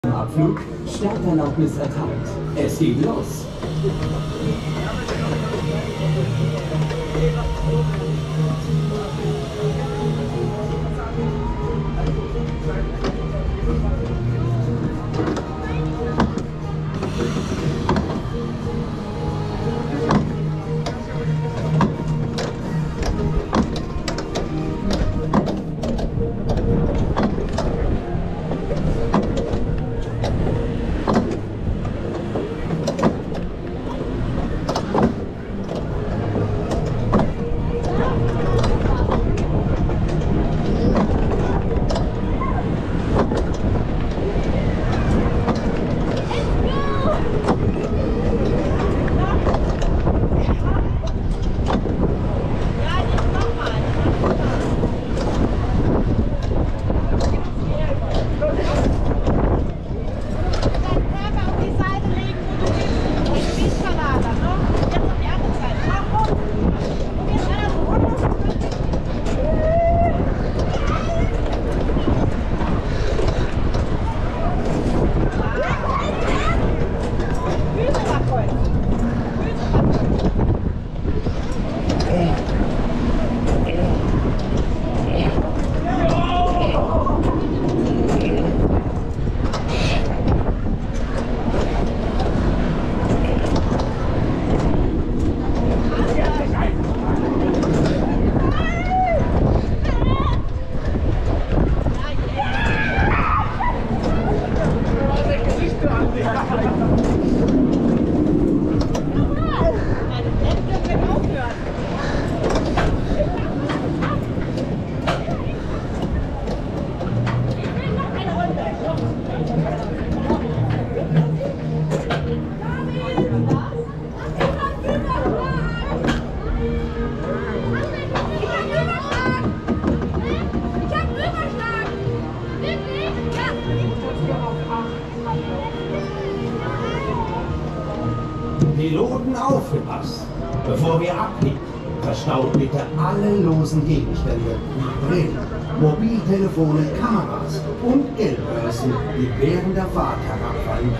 Sky Fly POV (4K 60FPS, 2024), Holiday Park Gerstlauer Sky Roller | Non-Copyright
Sky Fly is a Gerstlauer Sky Fly / Sky Roller at Holiday Park in Germany.  This thrill ride can be as wild or mild as the user likes.  If you rhythmically throw your weight around, you can get your gondola to invert quickly and often.